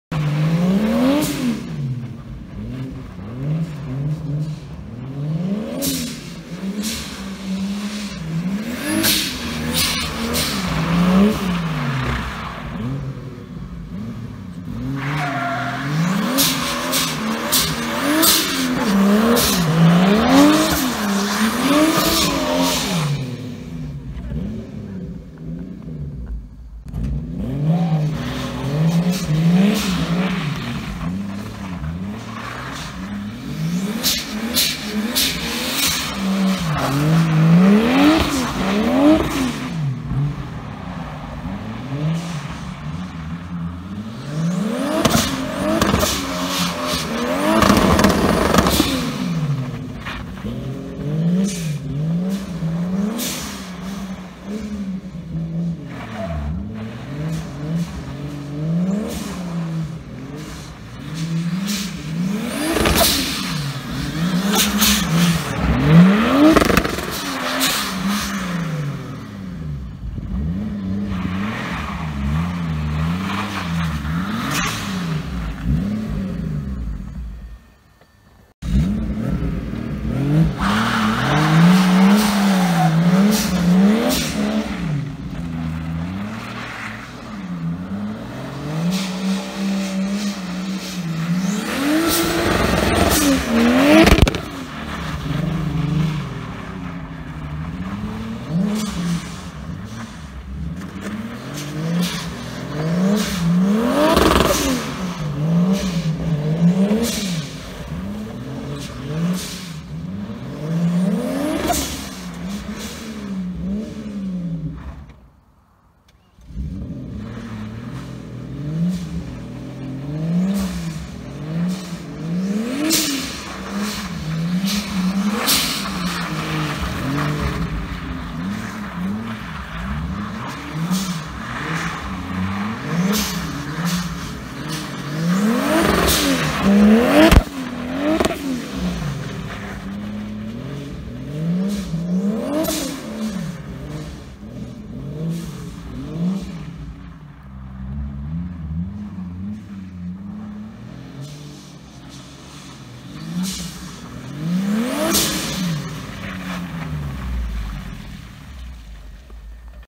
Volvo 245 M52B25 Turbo Donuts On Snow
Volvo 245
Swapped BMW M52B25
Garrett GT3076
VEMS
Winter 2023